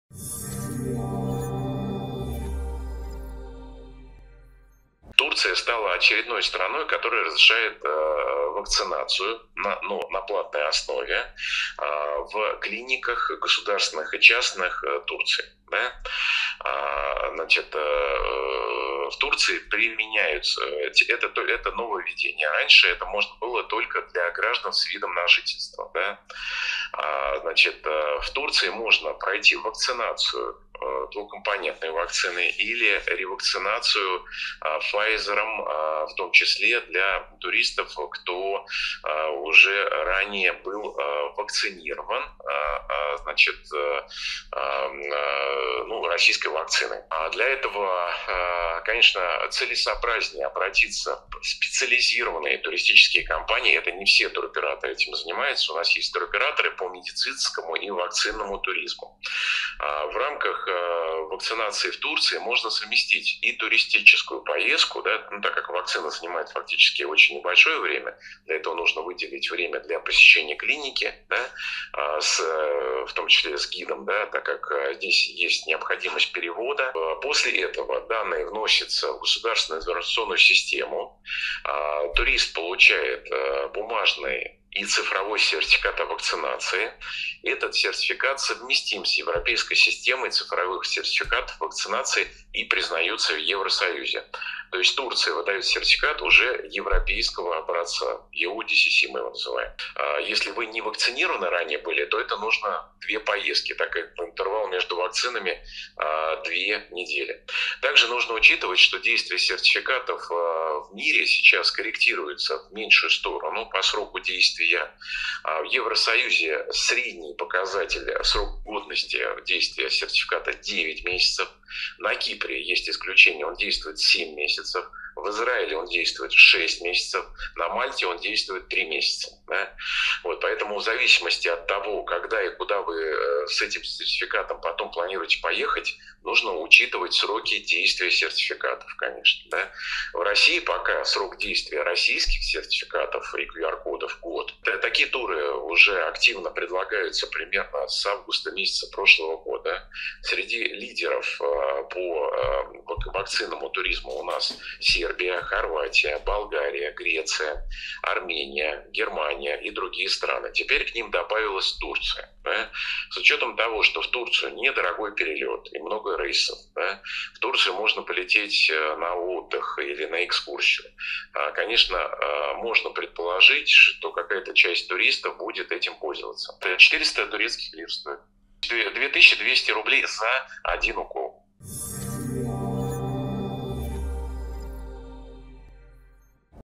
Названы условия и цена вакцинации в Турции: нужен переводчик
В список стран, где россияне могут получить зарубежную вакцину, добавилась Турция. Теперь можно совместить пляжный отдых с уколом зарубежной прививки, без которой нельзя посетить ряд других стран.

Дмитрий Горин, вице-президент Российского союза туриндустрии, озвучил главные правила получения искомого укола. Россиянам придется позаботиться о переводчике, который будет сопровождать их в клинику. Цена укола - 400 турецкий лир или около 2200 рублей.

Читайте свежие новости на сайте "МК"